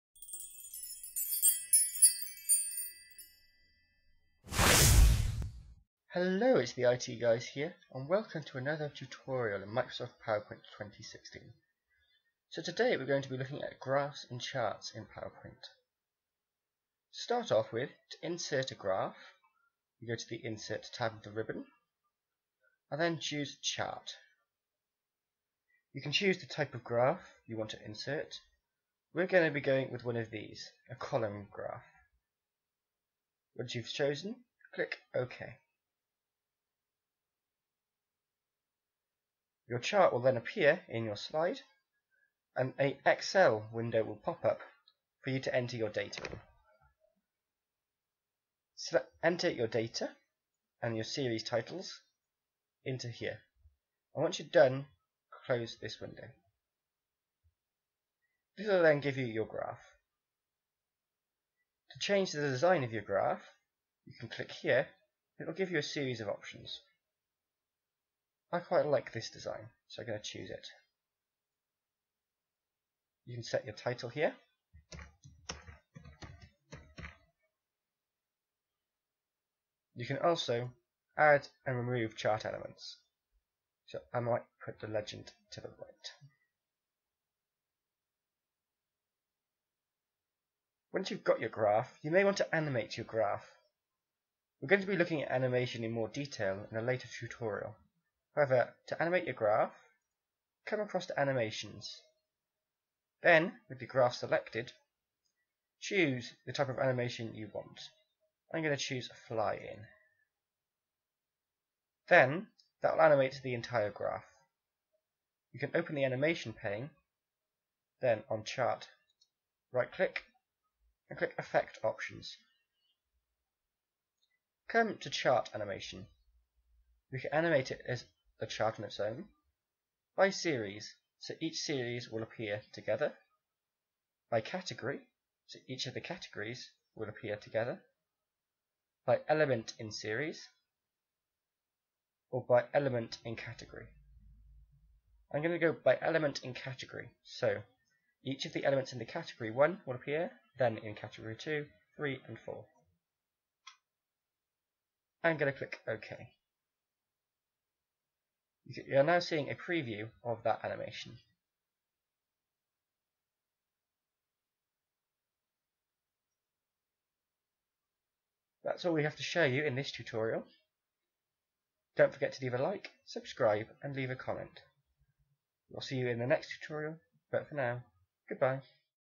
Microsoft PowerPoint 2016 Graphs and Charts Tutorial
In this tutorial, we show you how to create graphs and charts in your presentations in PowerPoint 2016. We also show you how to change the design of your graphs and charts. Finally we show you how to animate the elements of your graph.